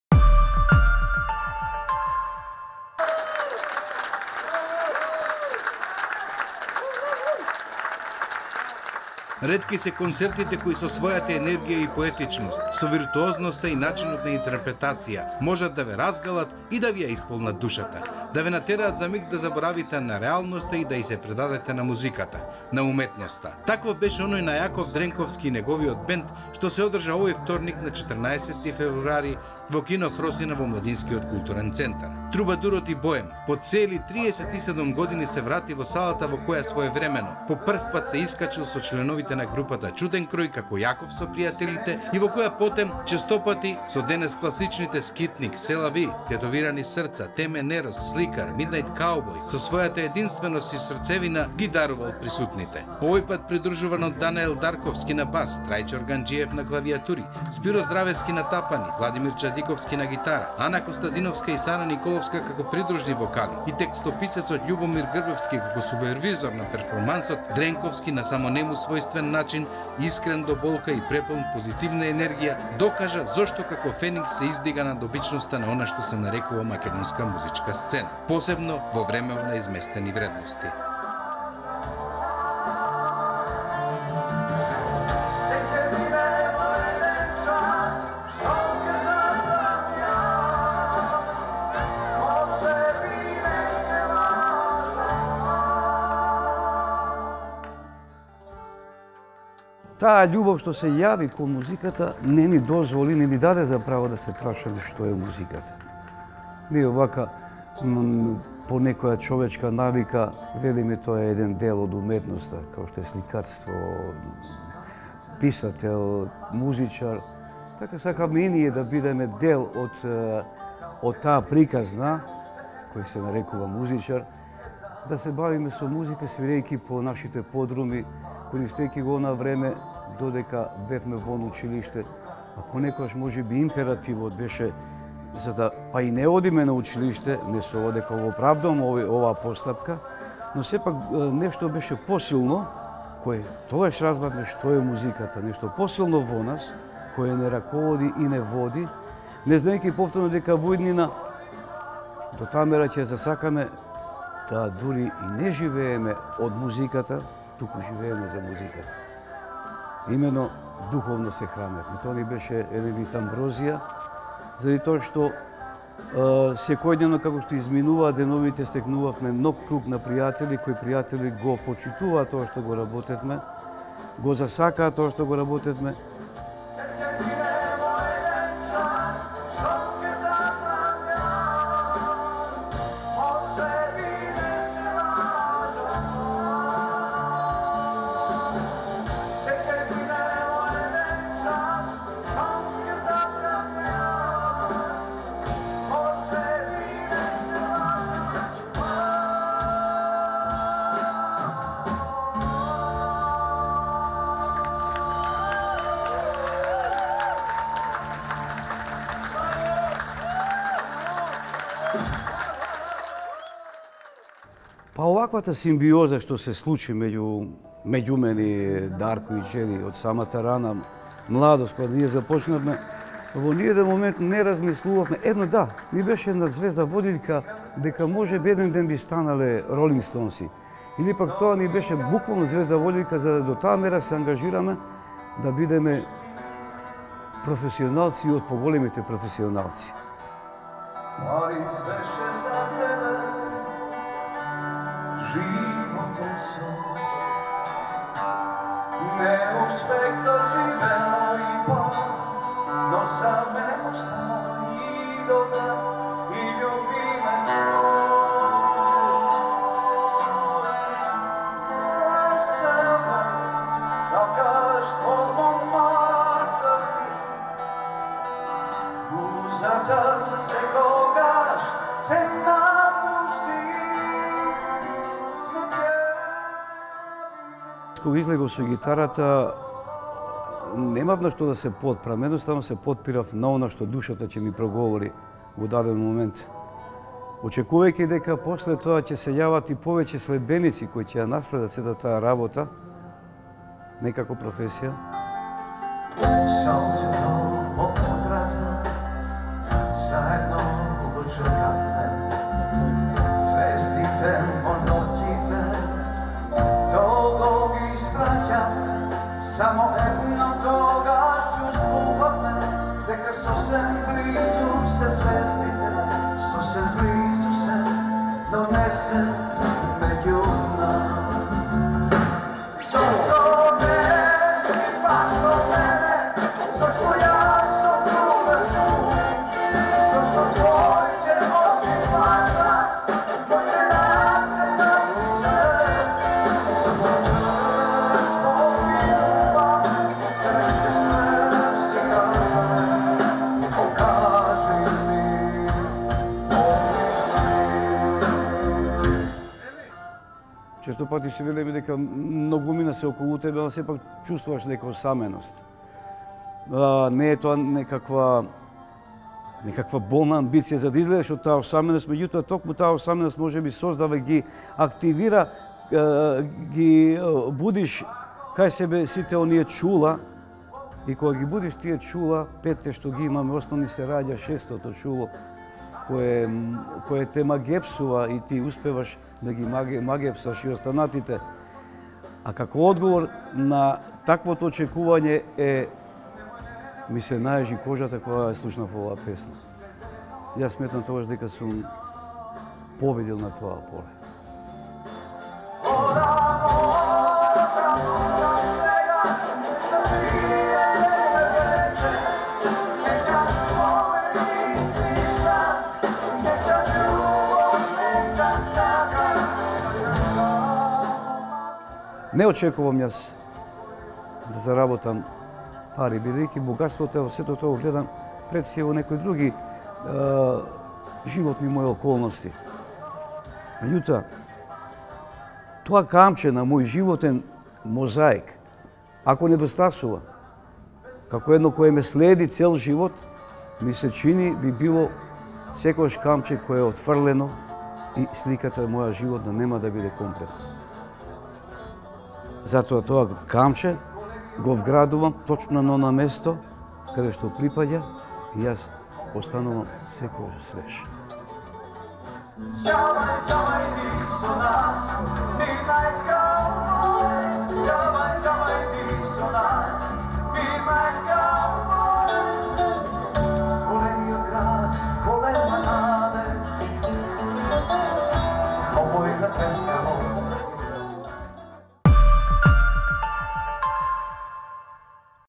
Полноќниот каубој во Кино Фросина во МКЦ
Ретки се концертите кои со својата енергија и поетичност, можат да ве разгалат и да ви ја исполнат душата. Таков беше оној на Јаков Дренковски и неговиот бенд што се одржа на 14 февруари во Младински културен центар во Скопје.